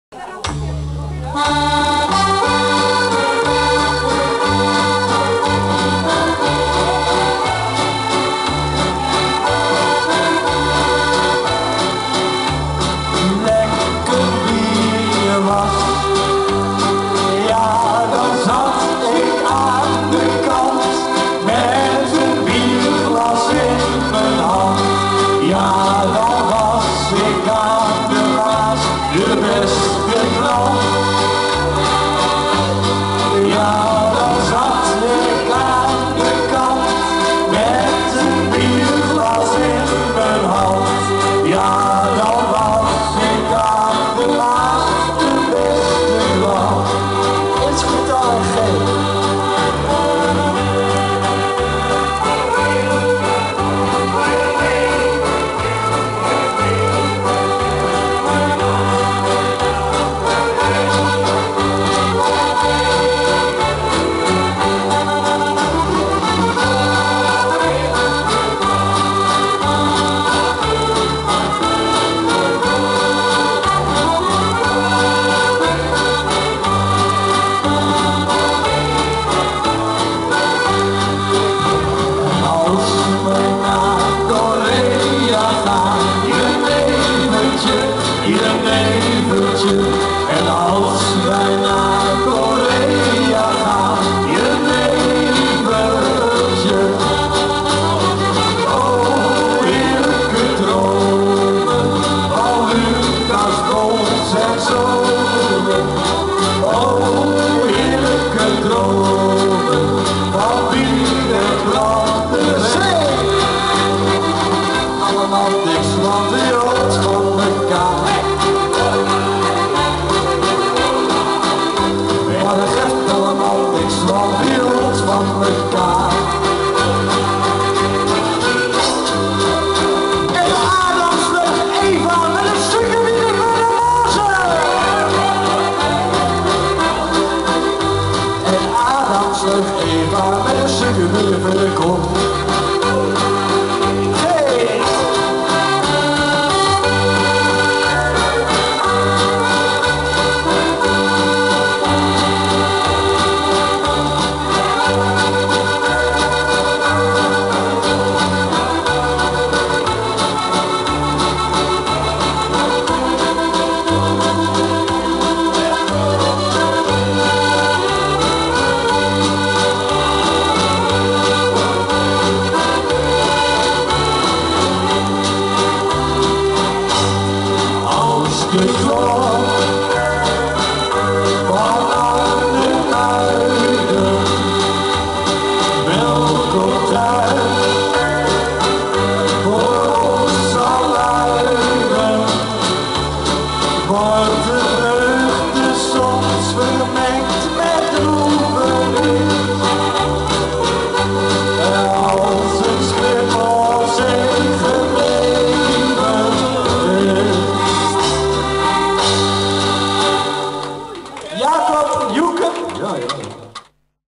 Wouter de Polkaman, Jacob (duo Nooitgedacht) & Joeke
Met Jacob en Joeke op visite bij Nelis en Leentje in de Woonboot nabij Emmercompascuum. Lekker ongedwongen spelen onder het genot van een pilsje!

Er komen geregeld muzikanten naar de Woonboot op zondagmiddag. Dit gezellige café bevind zich aan Kanaal A NZ 225 te Emmererfscheidenveen.